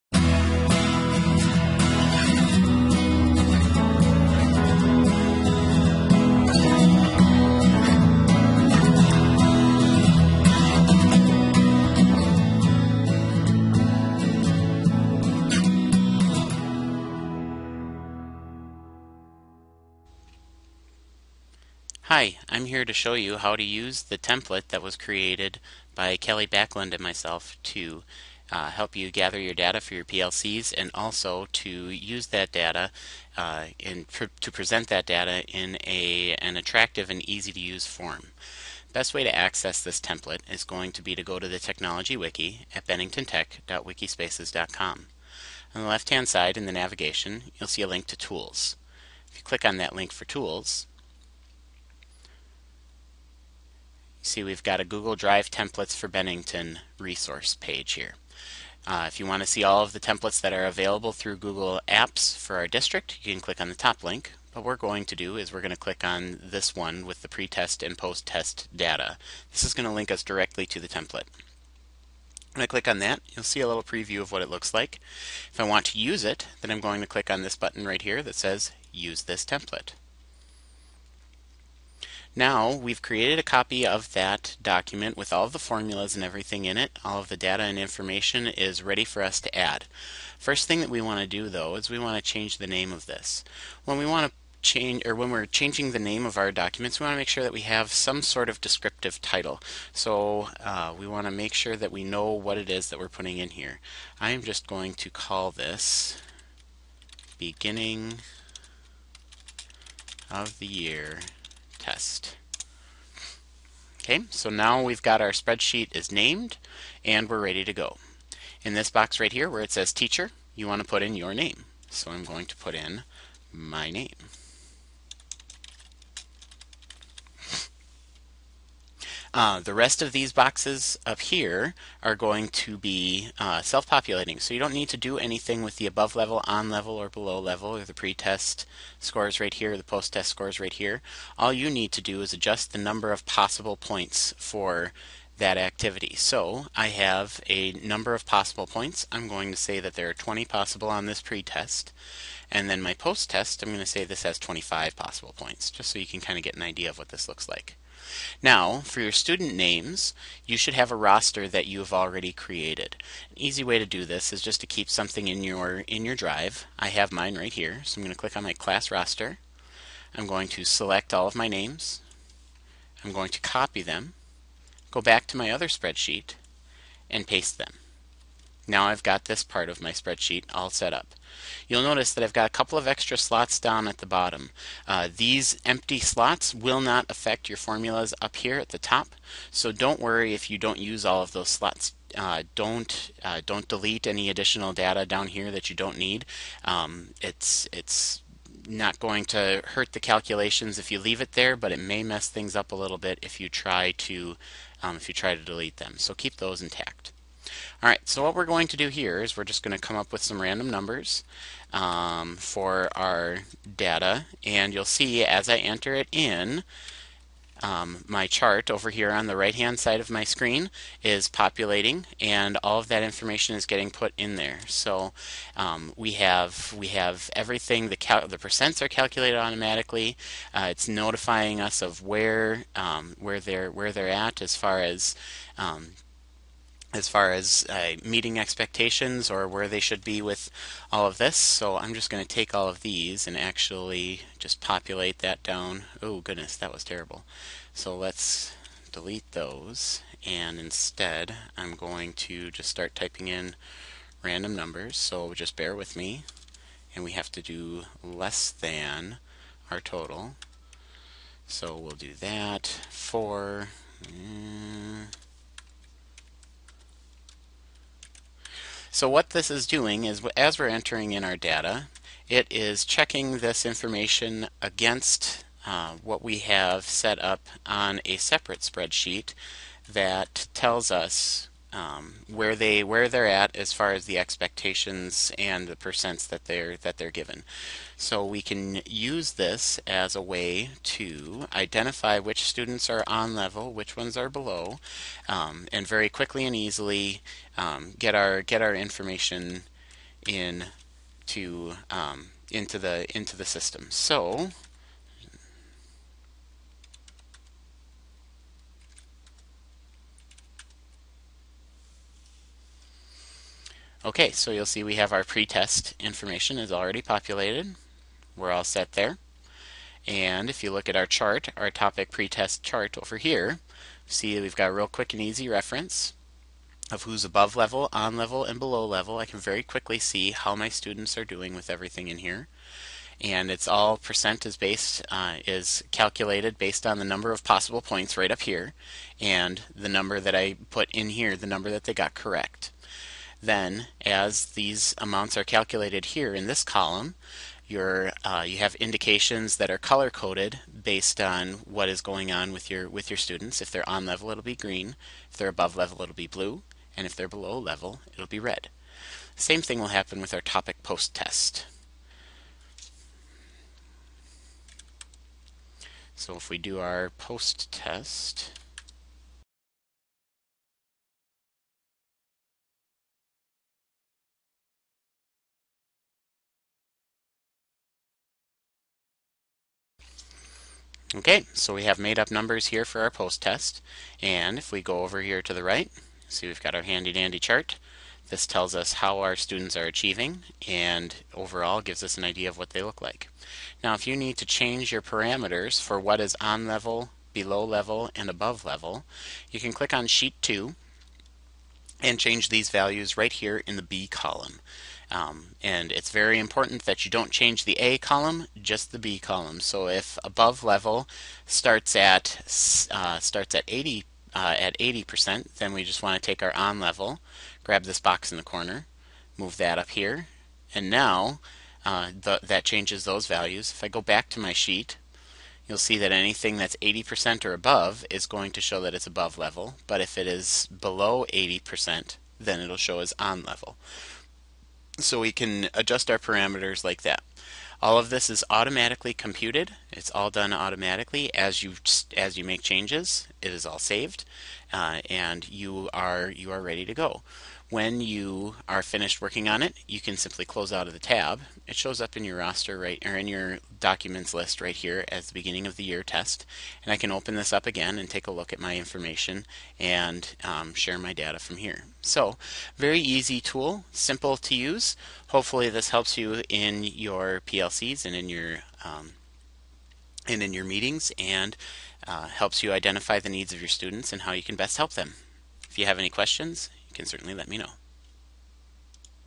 PLC Data Spreadsheet HowTo
Quick demonstration and explanation of how to use the Google Sheet template from Bennington's Technology Wiki to create a nice visual representation of student progress.
Here is a link to the template referenced in the video.